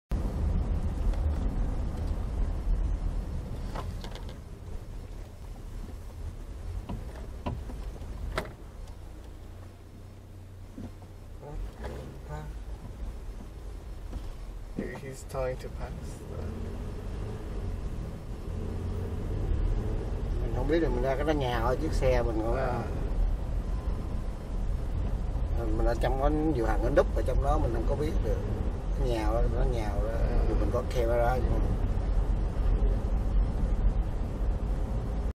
Skateboard
I was driving home late at night.  This is on Owensmouth St.  It seemed like this skateboarder tried to cause accident,  He saw my car and still stayed in middle of the street.  He even motioned me to pass but at the same time he moved further out.  I saw many other guys standing inside the curb too so I slow down and didn't pass him.
I suspected he tried to jump in front of my car and acted like I hit him and those guys inside will come out as witness.  He waited and then he probably saw my dash-cam so he moved back in and I passed it.
Be careful when you run into the similar situation, if you don't have a dashcam, use your phone to record it.